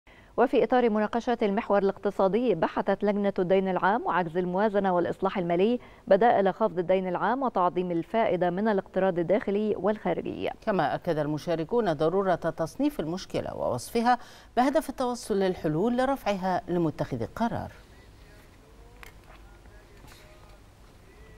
لجنةعجزالموازنة بالحوار الوطني:نستهدف الوصول لحلول علميةوعملية لمشكلةالدين العام لرفعهالمتخذي القرار
نشرة التاسعة | لجنة عجز الموازنة بالحوار الوطني: نستهدف الوصول لحلول علمية وعملية لمشكلة الدين العام لرفعها لمتخذي القرار
نشرة _التاسعة